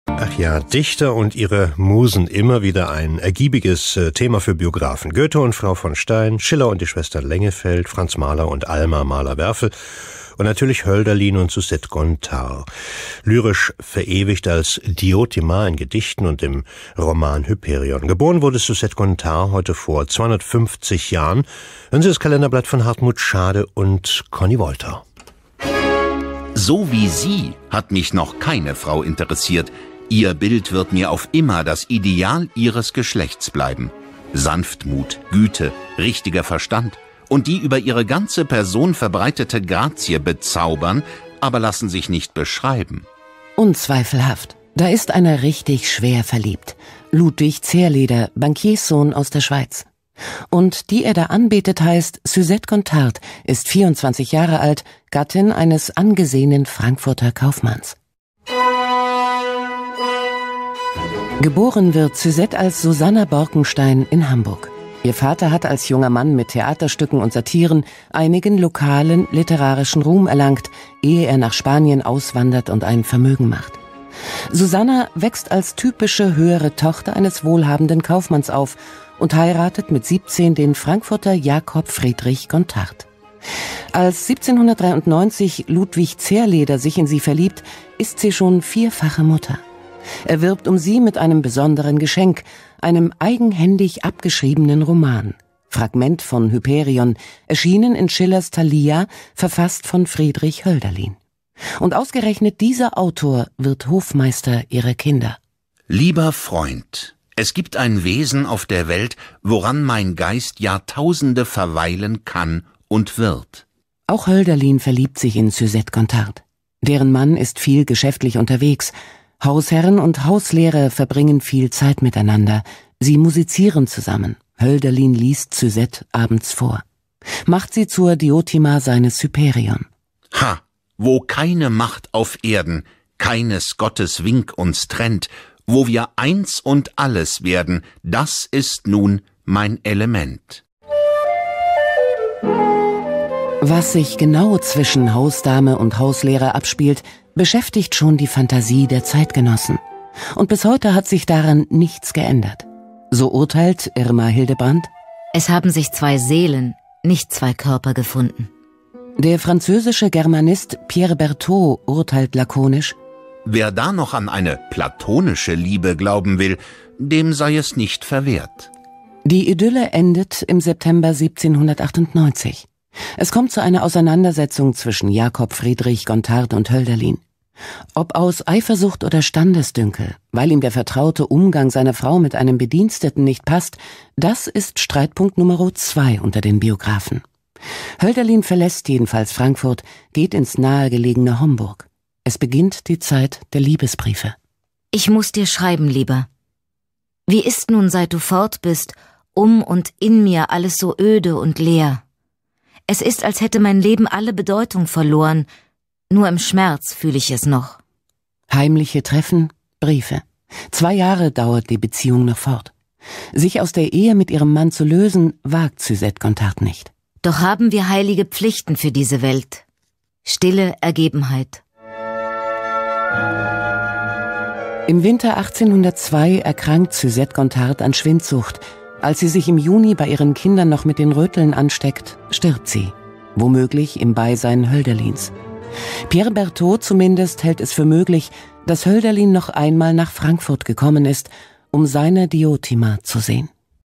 MDR 09.02.1769 Susette Gontard geboren
Als "Diotima" wurde sie weltberühmt: die Kaufmannsgattin Susette Gontard, in deren Haus Hölderlin Hauslehrer war und mit dem sie ein Verhältnis verband, über dessen Natur bis heute spekuliert wird. Von Hartmut Schade.